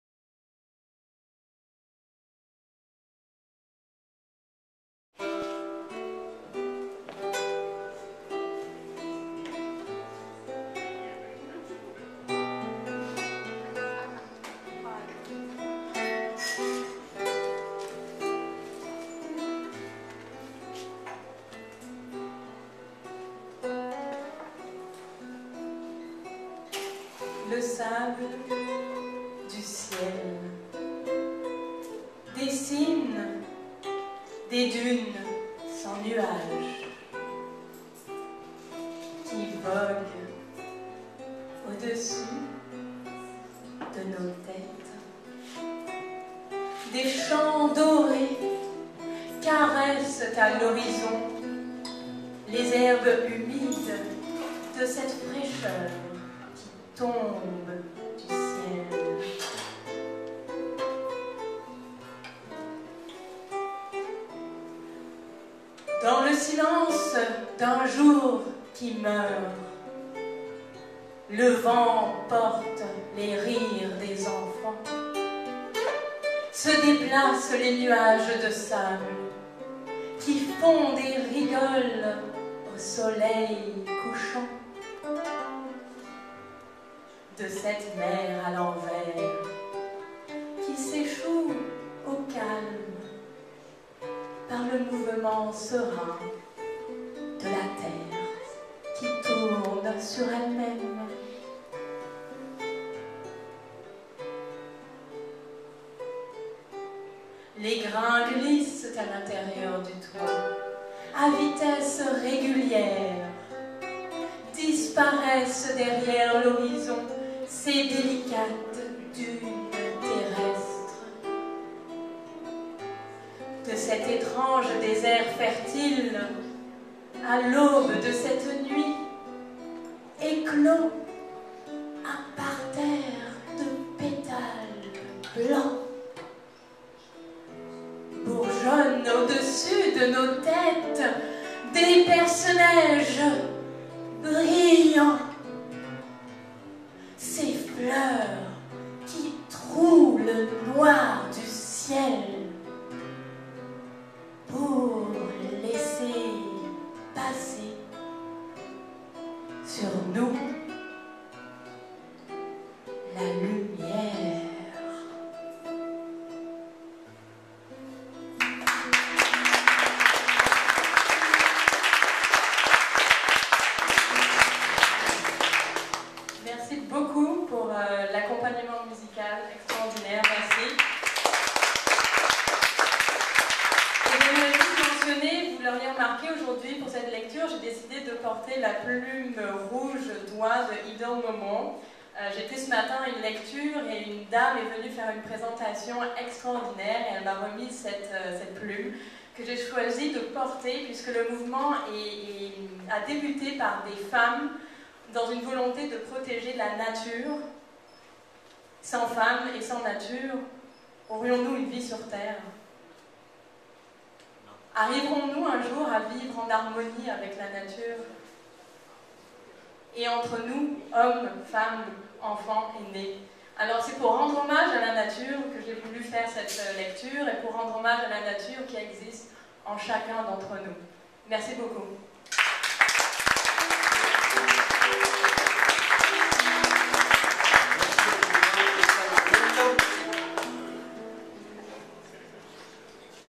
''LE SABLE DU CIEL'' de Tessa Manuello, Poète invitée à la Cour des Poètes
Lecture de poésie de Tessa Manuello, Poète
À La Cour des Poètes de Normand Lebeau et Roger Michaël
Cabaret Mado, Montréal, 10 mars 2013